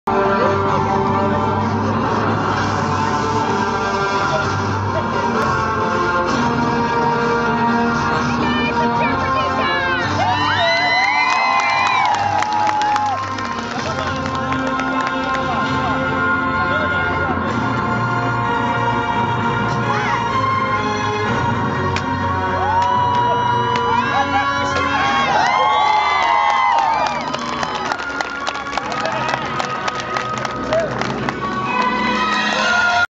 Master Rim Tae Kwon Do at Airdrie Fest 2012 - AIR 106.1 FM Performance Zone
Students from Master Rim Tae Kwon Do performed during Airdrie Fest on September 8, 2012 in the AIR 106.1 FM Performance Zone.